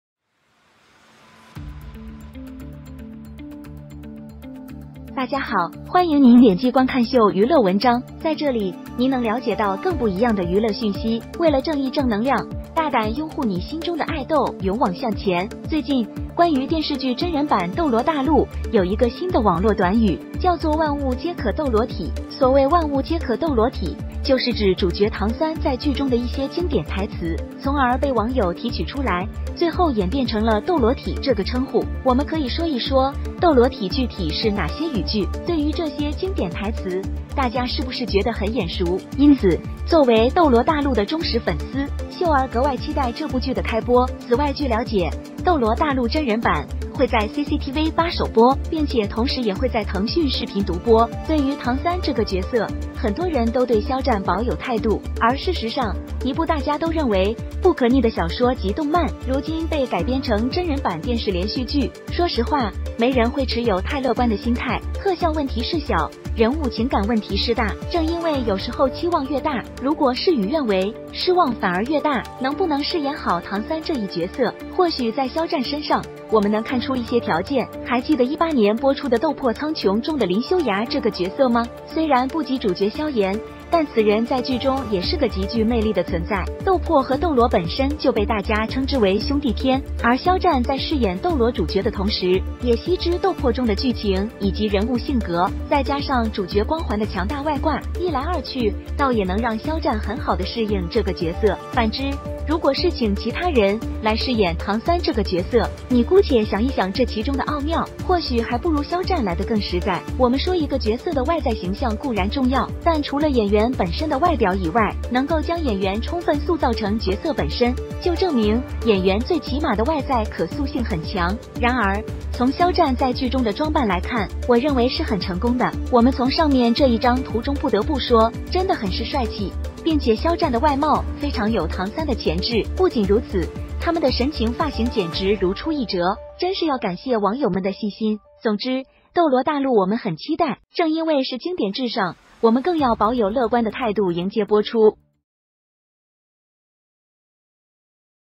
“斗羅體”梗被玩壞，肖戰飾演男主的2個合理解釋-"Douluo body" stalk is badly played, 2 reasonable explanations for Xiao Zhan
對於唐三這個角色，很多人都對肖戰保有態度。而事實上，一部大家都認為不可逆的小說及動漫，如今被改編成真人版電視連續劇，說實話，沒人會持有太樂觀的心態。
For the role of Tang San, many people have an attitude towards Xiao Zhan. In fact, a novel and anime that everyone considers irreversible has now been adapted into a live-action TV series. To be honest, no one has a too optimistic attitude.